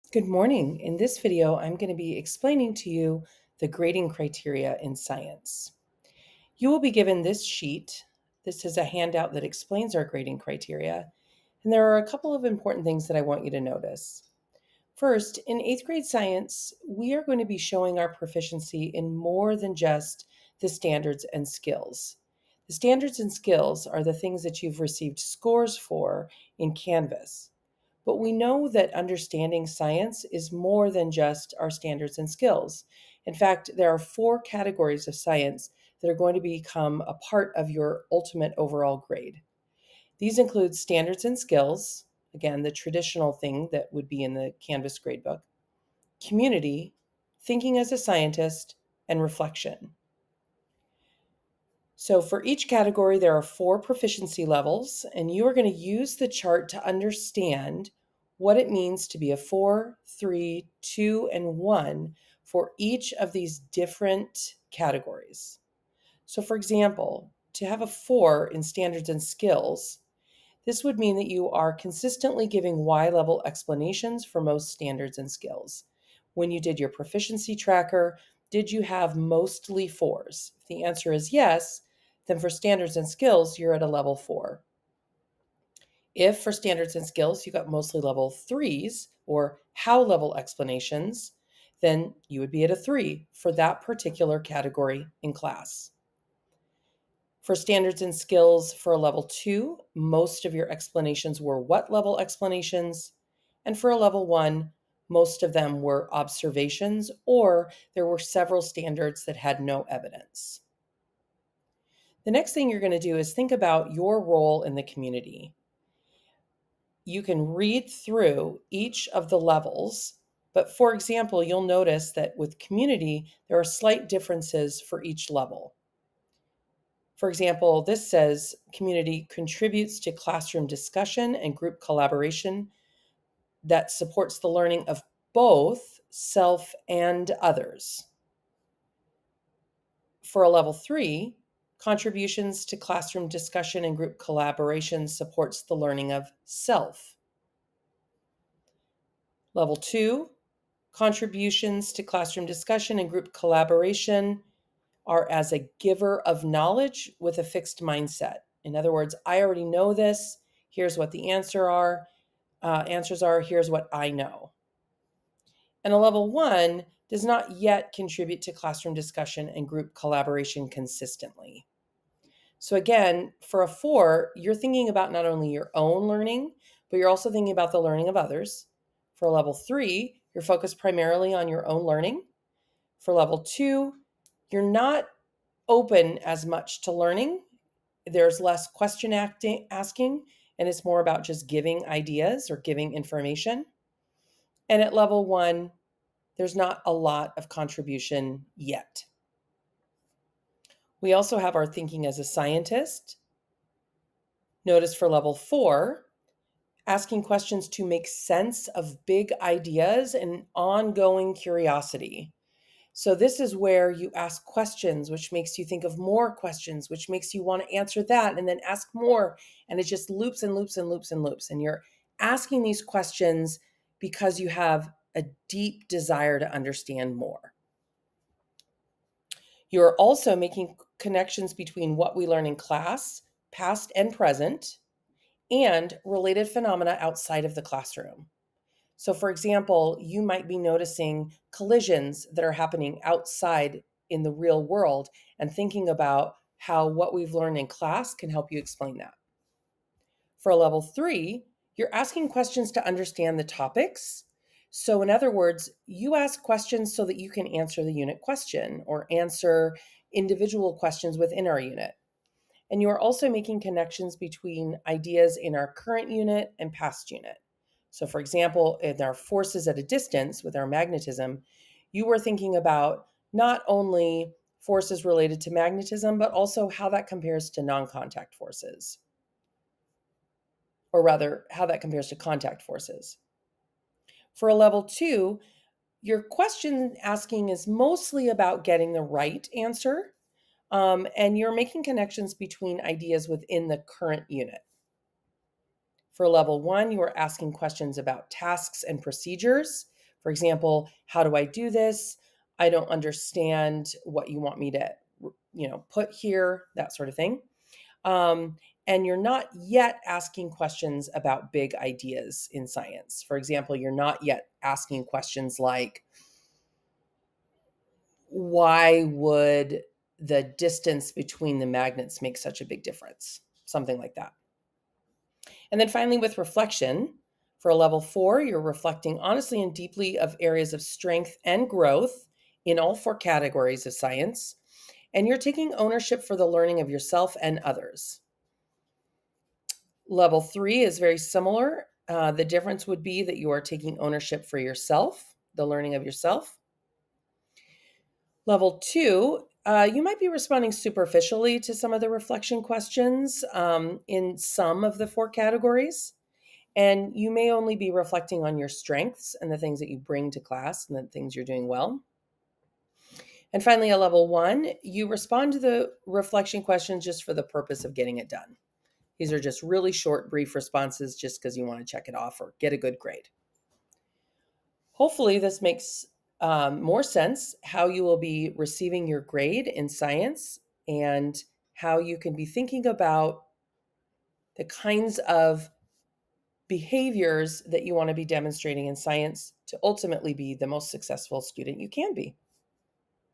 Grading Criteria for 8th Grade Science EXPLAINED
In this video, I will explain the Four Categories of Science Excellence and how these criteria align with 4 proficiency levels.  Students will use this information to evaluate their proficiency in each category and ultimately propose a final grade in 8th-grade science.